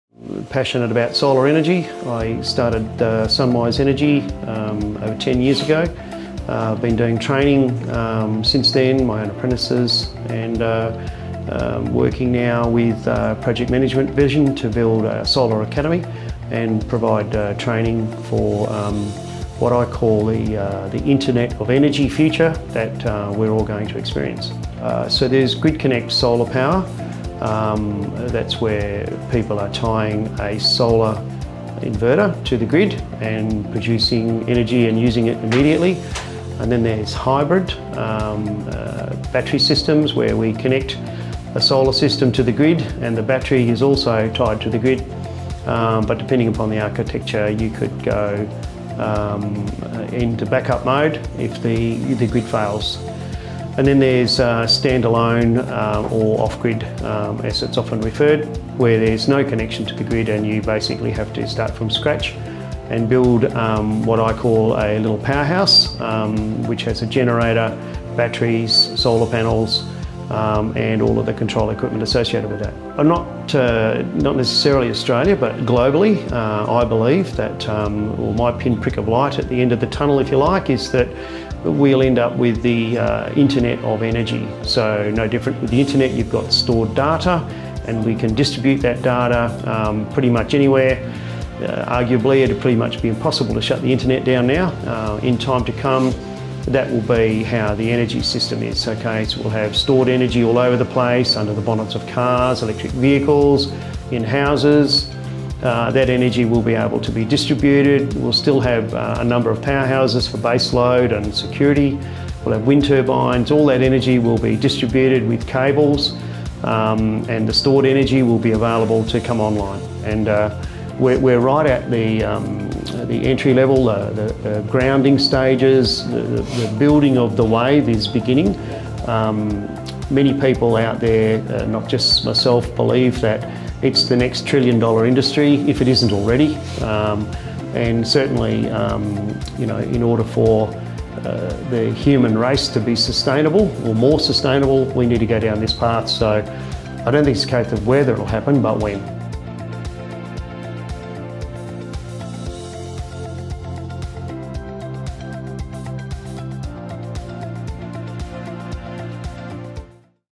Trainer Talk - Glen Holland
Glen is a Licensed Electrical Contractor and an ‘A’ Grade Electrician by trade, by trade, with over forty years of experience in the mining and solar energy industry. Glen is a specialist in the renewable energy field, with over fifteen dedicated years of experience in the design and installation of solar power, energy/ battery storage, solar pumping and general electrical.

Glen is the founder and owner of one of Western Australia’s oldest solar organisation, SunWise Energy. His company has installed over 30MW of solar power, representing approximately 2% of WA’s solar power.

As an active participant in the renewable energy industry, Glen is in the know of the latest industry trends, making him a great mentor and trainer.

For more information about PMV's renewable energy courses.

RTO 51178 | RTO 52698